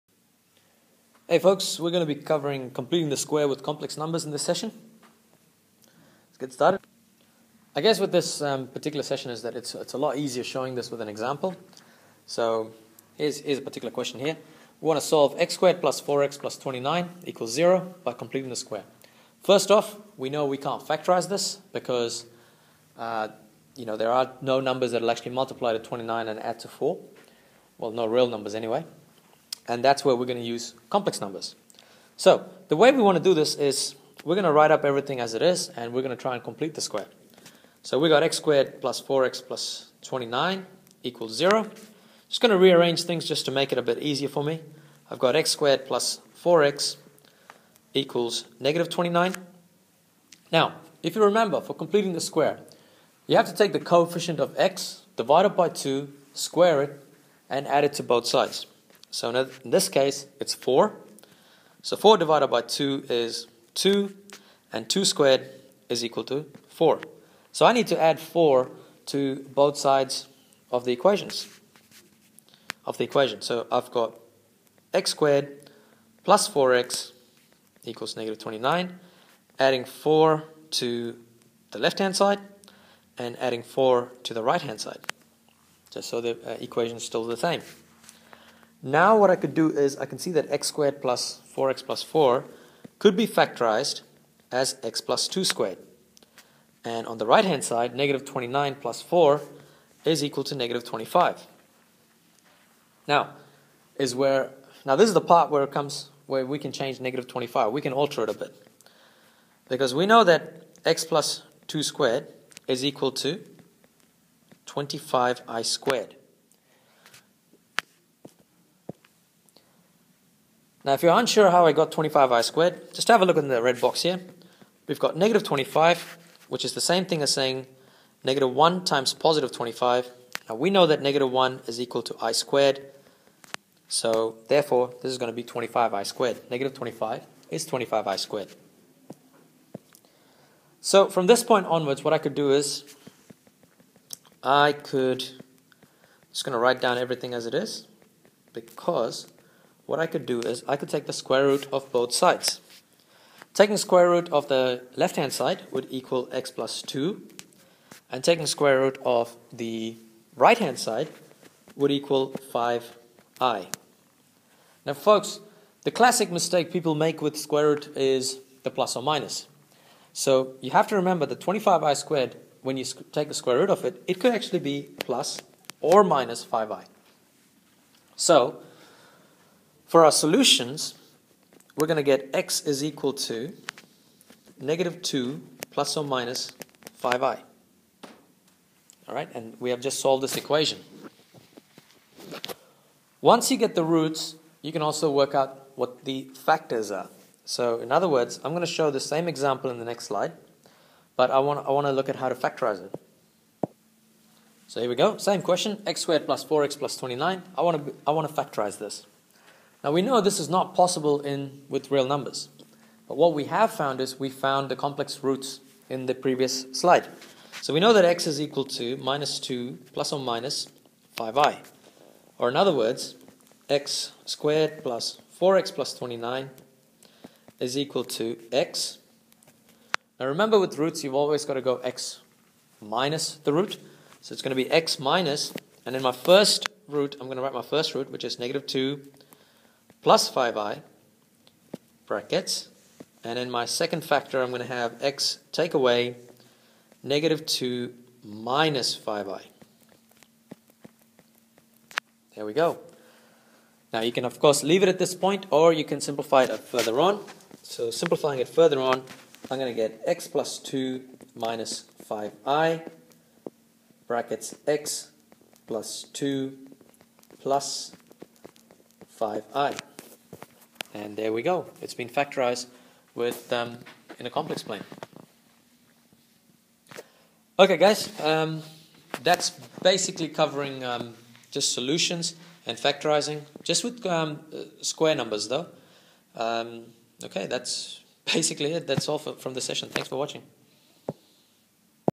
Completing the Square with Complex Numbers (2014 Edition)
NCEA Level 3 Calculus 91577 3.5 Algebra & Complex Numbers Skills (2014) Delta Ex 12.05 P224 "1 to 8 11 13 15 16"